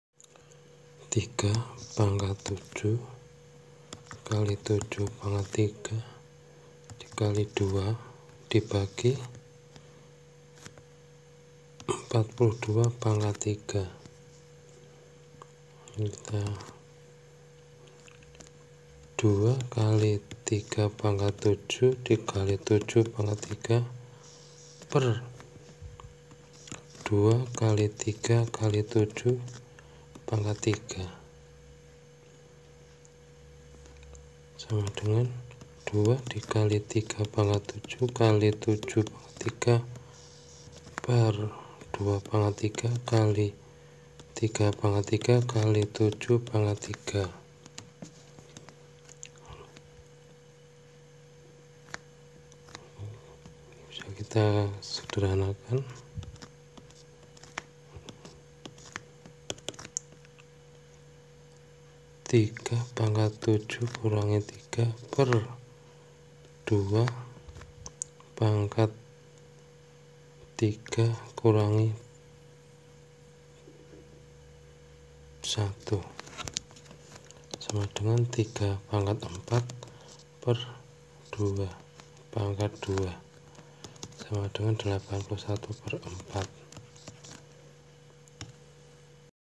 3 pangkat 7 kali 7 pangkat 3 kali 2 dibagi 42 pangkat 3 =
- Bentuk pertanyaan 3 pangkat 7 kali 7 pangkat 3 kali 2 dibagi 42 pangkat 3=